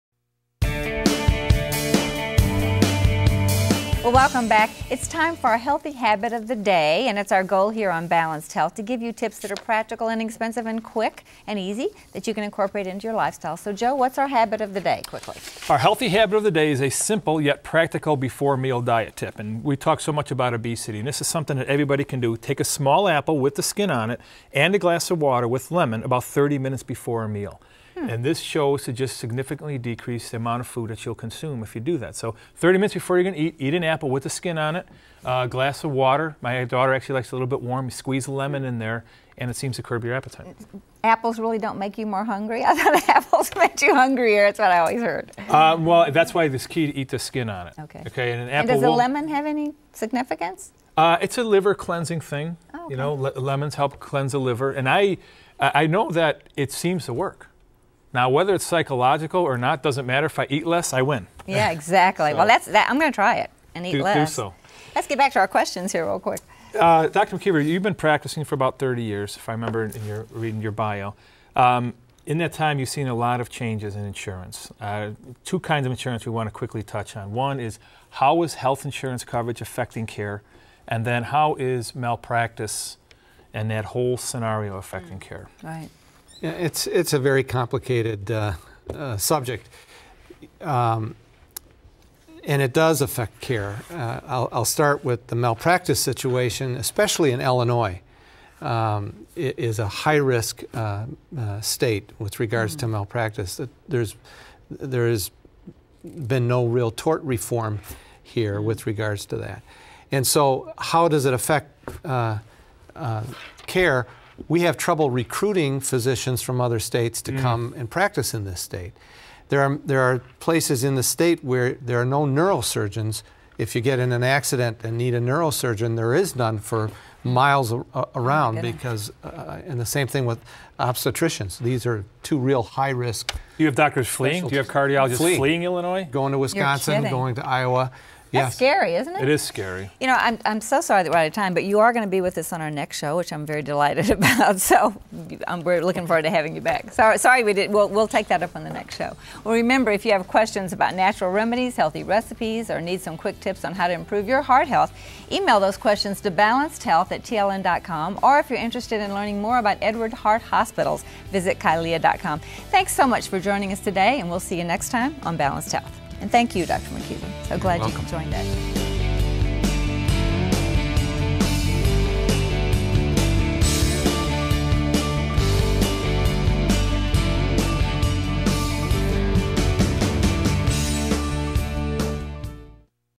Questions on Heart Health Insurance Coverage Affecting Care?
Questions on heart health; does insurance coverage affect cardiology care?  Interview with Cardiologist Dr. Louis McKeever  healthy eating tips; lemon for liver-cleansing.